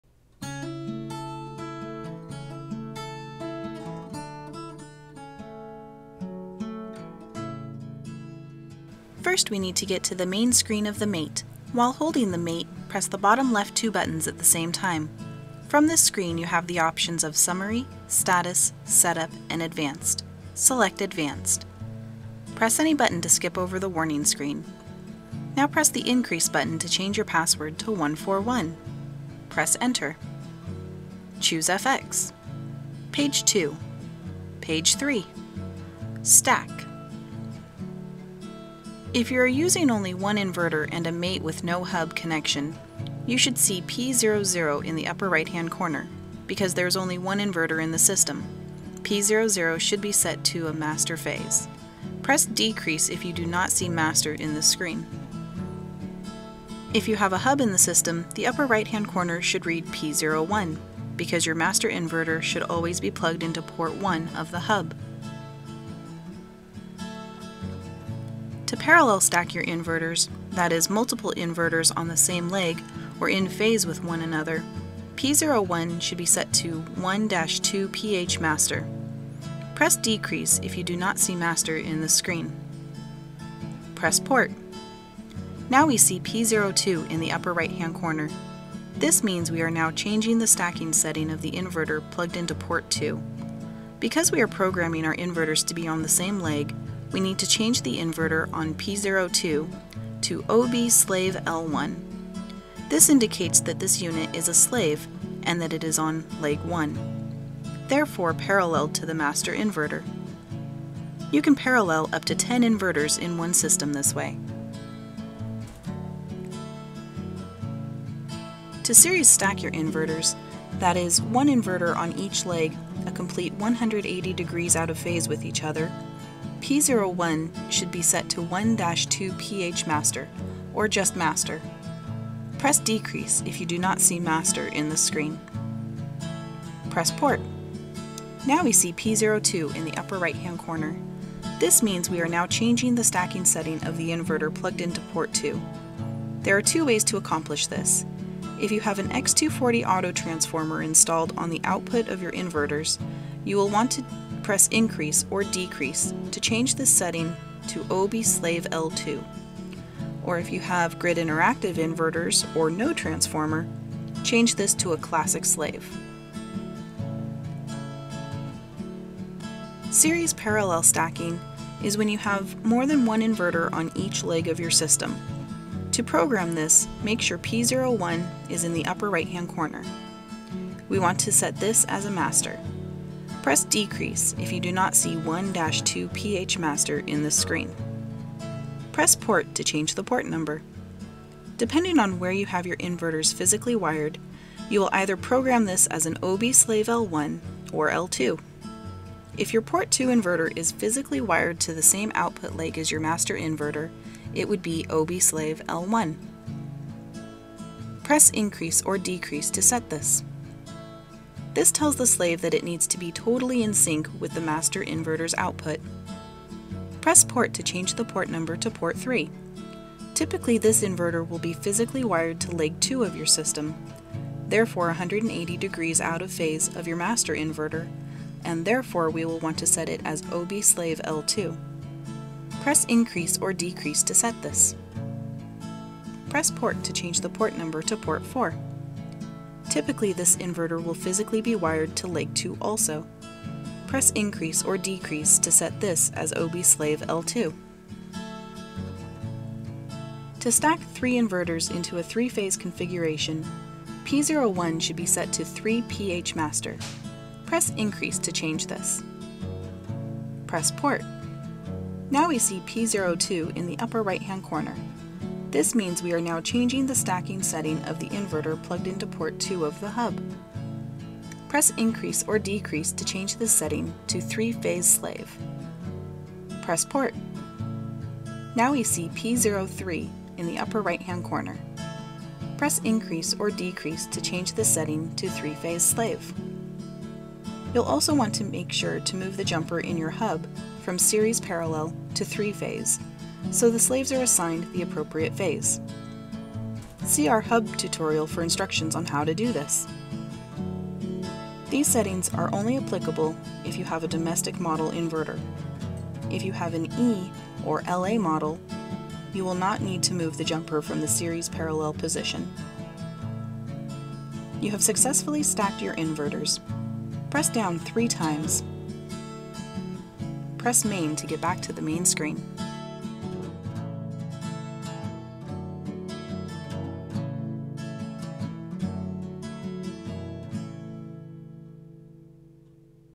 Tech Support | Inverter Stacking
Stacking allows all the FX's to work together as a single system. Stacking assigns the FX's to power individual legs of the system, an Outback MATE is required to recognize and program the Fx's, each needs to be assign a status-Master or Slave. The master is the primary and most heavily used unit, slave will assist as demand increases.